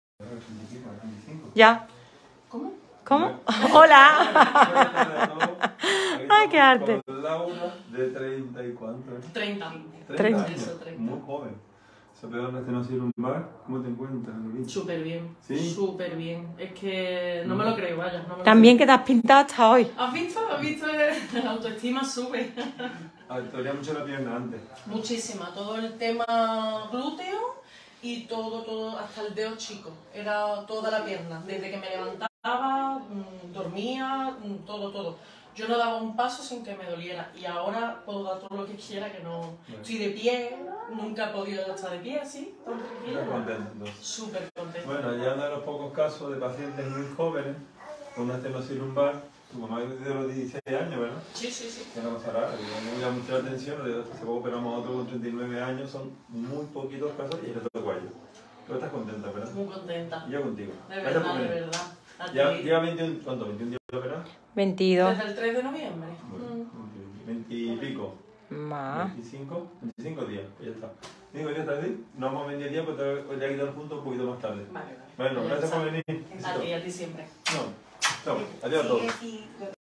Laura después después de 25 días - Clínica Dr. Mosqueira
Así se encuentra Laura a 25 días de su intervención quirúrgica, por una estenosis lumbar.